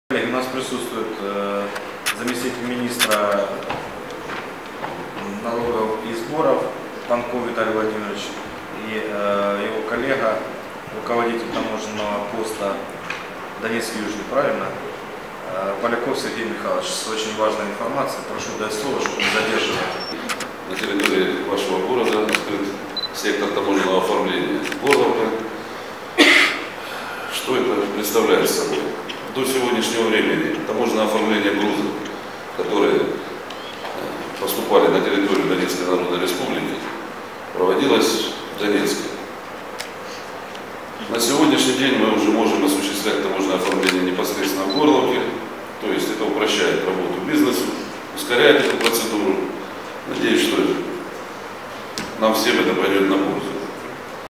В Горловке открыли Сектор таможенного оформления
Открытие Сектора таможенного оформления в Горловке